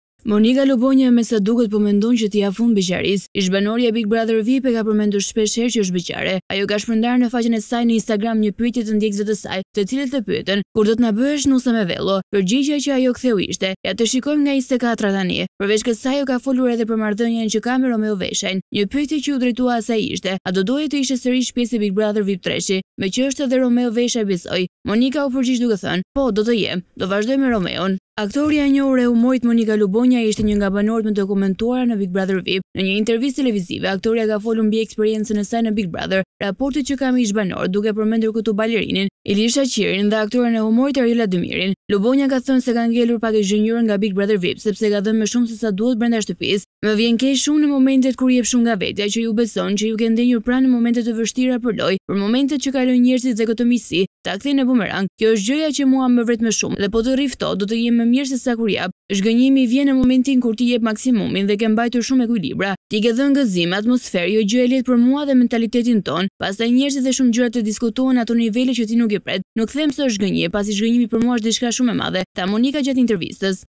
Monika Lubonja vendos t’i japë fund beqarisë? Zbulon datën
Monika Lubonja, mesa duket po mendon që t’i japë fund beqarisë.
Ish-banorja e “Big Brother Vip”, e ka përmenduar shpesh herë që është beqare. Ajo ka shpërndarë në faqen e saj të Instagramit një pyetje të ndjekësve të saj të cilët e pyetën: “Kur do t’na bëhesh nuse me vello?”
Përgjigja që ajo ktheu ishte: “Ja të shikojmë nga 24 tani.”
Përveç kësaj ajo ka folur edhe për marrëdhënien që ka me Romeo Veshajn.
Një pyetje që iu drejtua asaj ishte: “A do doje të ishe sërish pjesë e ‘Big Brother Vip 3’ meqë është dhe Romeo Veshaj besoj?”
Monika u pergjigj duke thene: Po të jem, do vazhdoj me Romeon
Aktorja e njohur e humorit, Monika Lubonja ishte një nga banoret më të komentuara në Big Brother Vip. Ne nje interviste televizive, aktorja ka folur mbi eksperincën e saj në Big Brother, raportet që ka me ish-banorët duke përmendur këtu balerinin, Ilir Shaqiri dhe aktoren e humorit, Arjola Demiri.
Lubonja ka thënë se ka ngelur pak e zhgënjyer nga Big Brother Vip sepse ka dhënë më shumë seç duhet brenda shëtpisë.
‘’Më vjen keq shumë në momentet kur jep shumë nga vetja, që iu beson, që iu ke ndenjur pranë në momente të vështirë për lojë, për momentet që kalojnë njerëzit dhe këtë miqësinë ta kthejnë në boomerang. Kjo është gjëja që mua më vret më shumë dhe po të rri ftohtë, do jem më mirë sesa kur jap. Zhgënjimi vjen në momentin kur ti jep maksimumin dhe ke mbajtur shumë ekuilibra. Ti ke dhënë gëzim, atmosferë jo që e lehtë për mua dhe mentalitetin tonë. Pastaj njerëzit dhe shumë gjëra të diskutohen në ato nivele që ti nuk pret. Nuk them se është zhgënjim, pasi zhgënjimi për mua është diçka shumë e madhe’’, tha Monika gjatë intervistës.